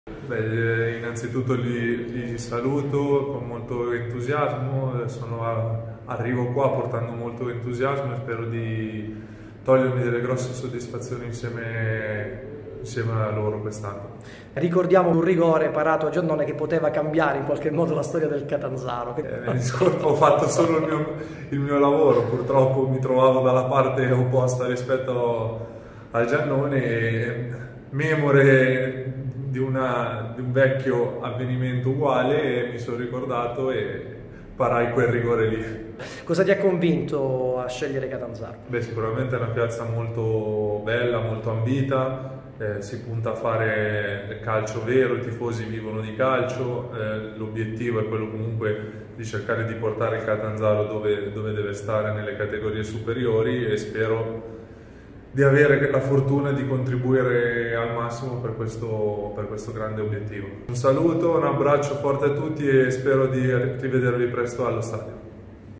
Welcome | Le parole del nuovo arrivo Branduani
Il nuovo portiere biancorosso si presenta ai tifosi del Catanzaro.